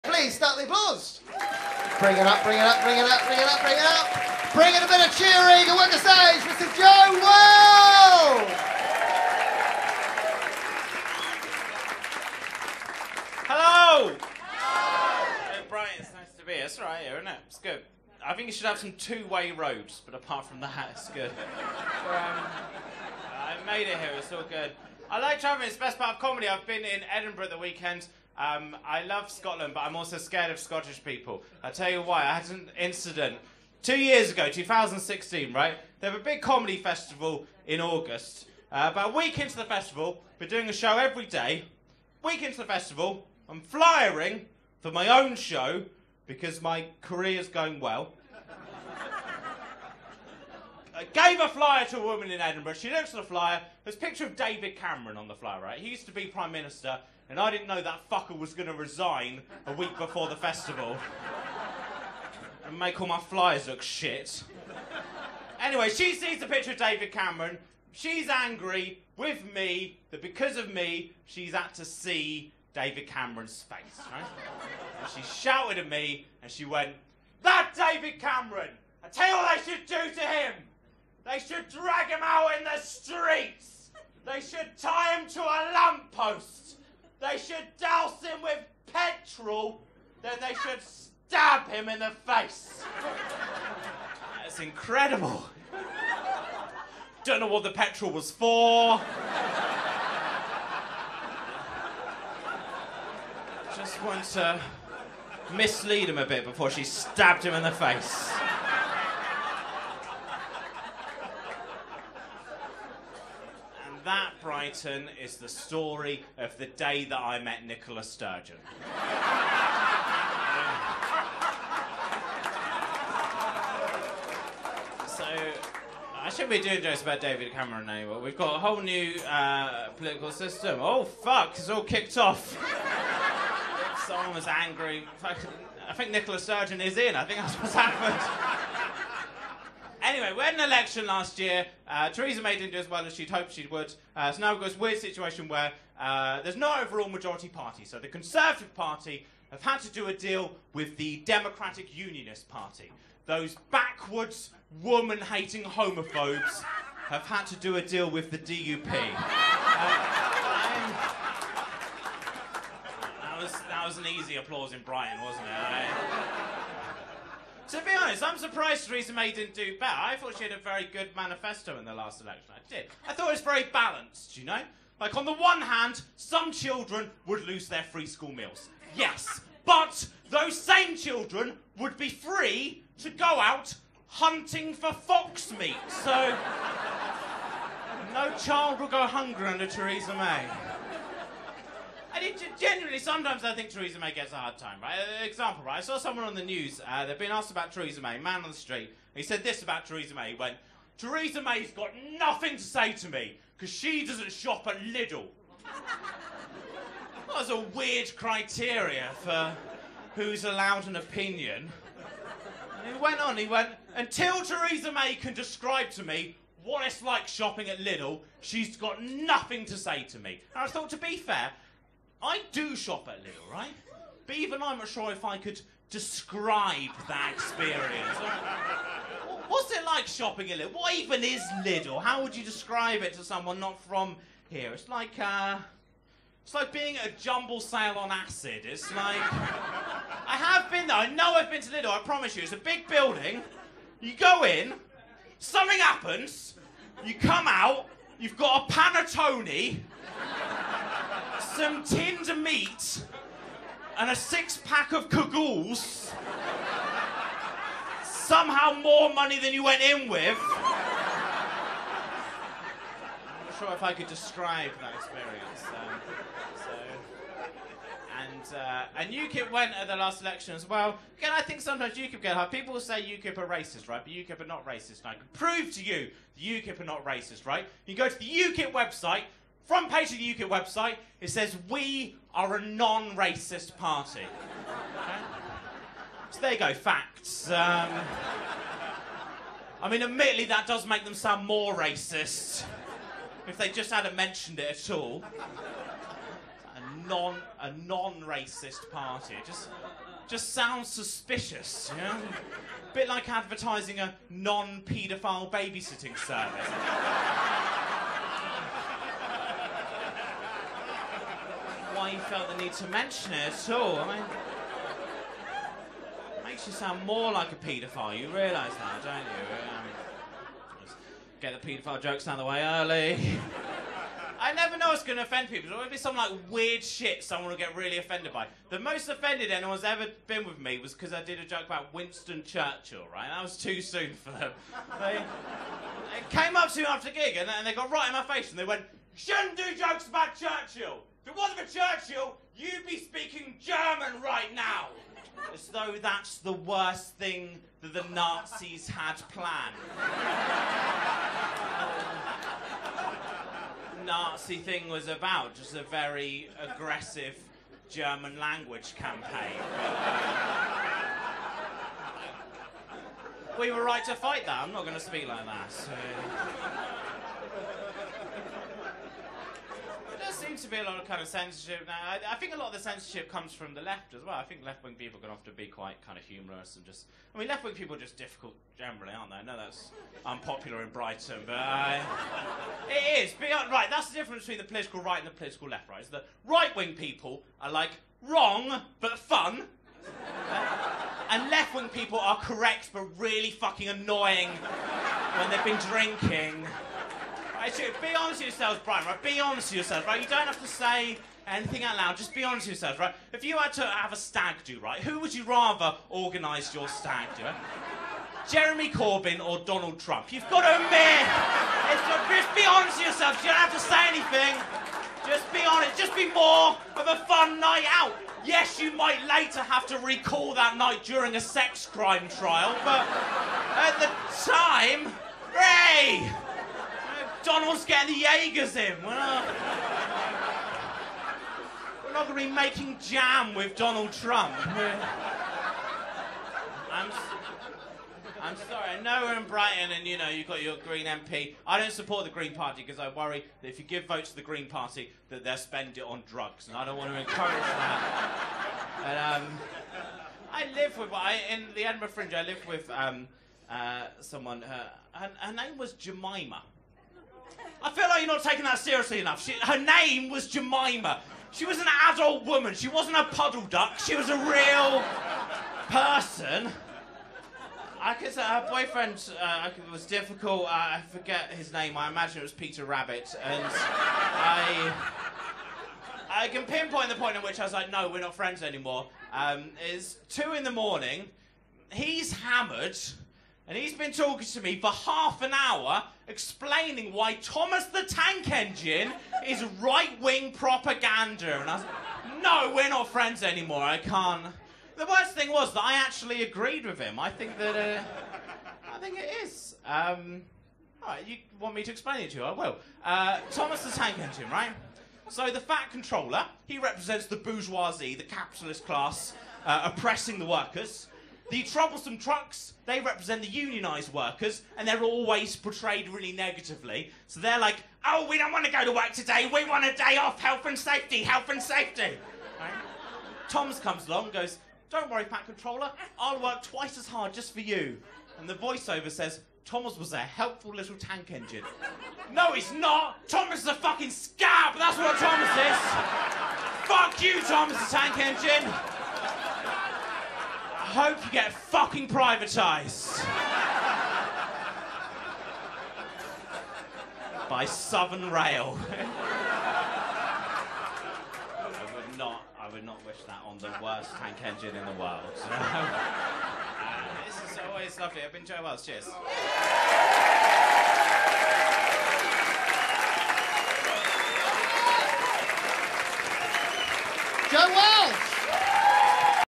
Joe Wells at Comic Boom 2018
a short set at the Komedia. If you like what I do I have CDs and Downloads at joewells.bandcamp.com